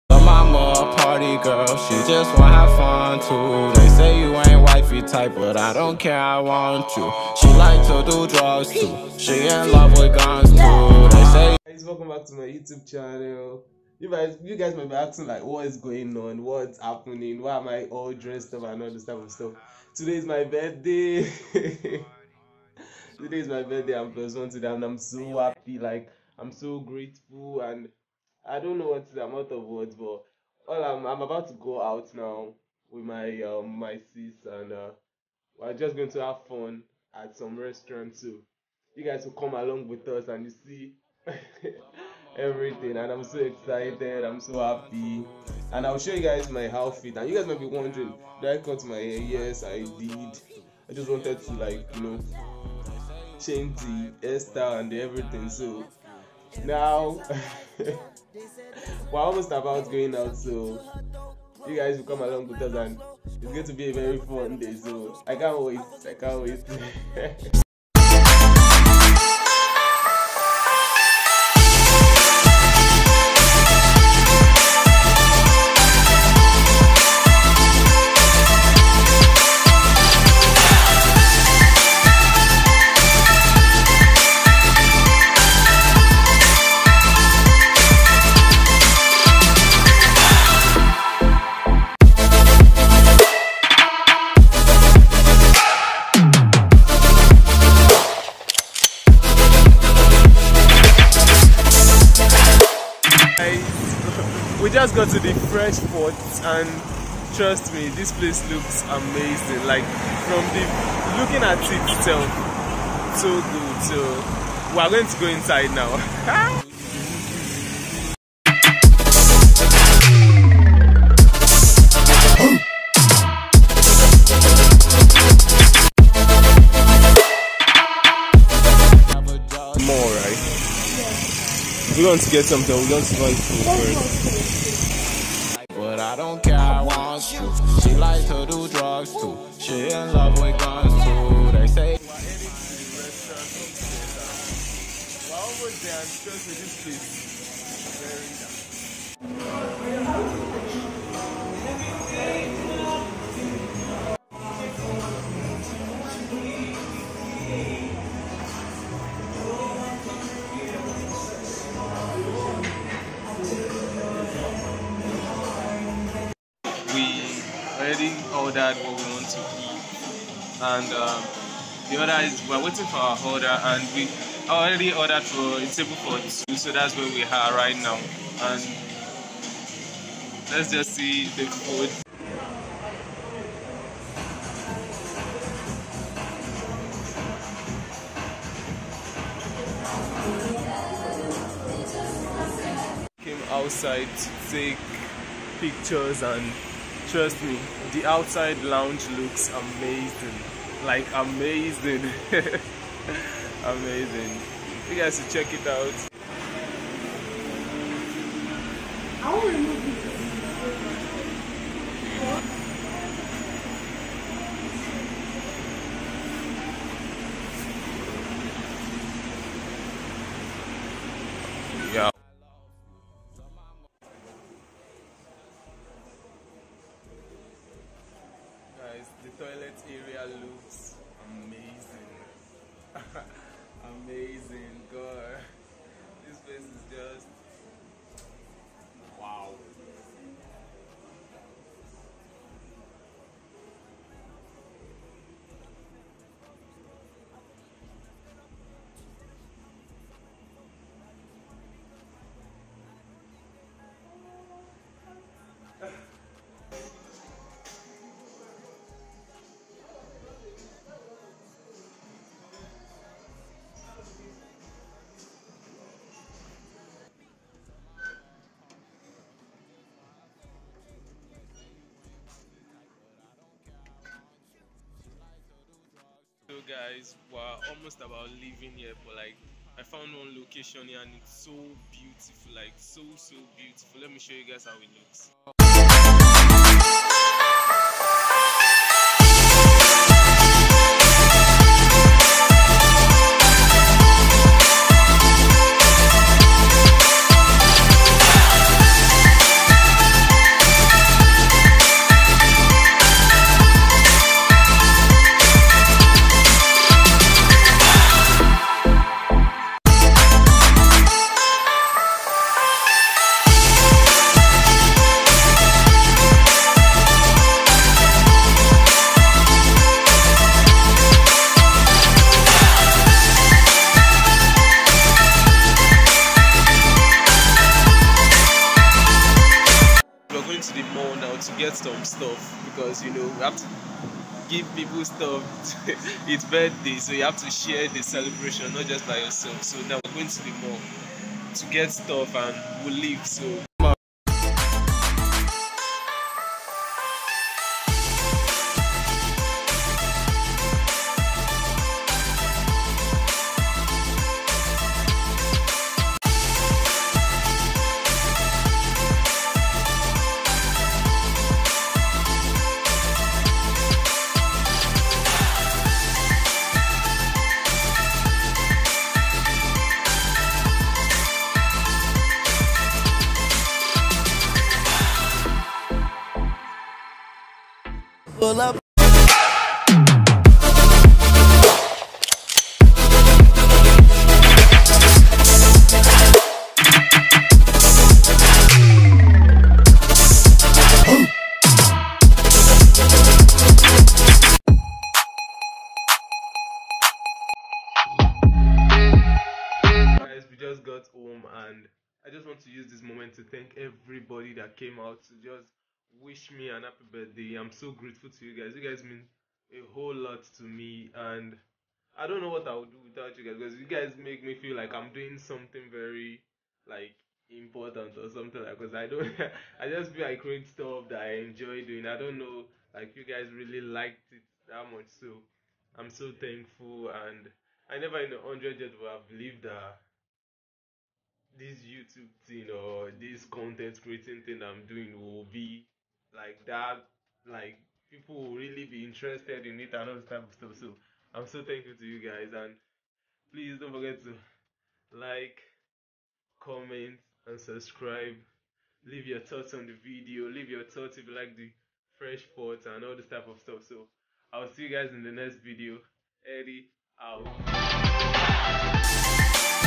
MY BIRTHDAY VLOG| FRESHFORTE RESTAURANT| EDDIE'S VLOG#4
THANKS AGAIN FOR THE WISHIES GUYS, freshforte is just an amazing restaurant, i had alot of fun, guys pls share this video with your fams it helps the channel grow also leave your thought down below and subscribe thanks.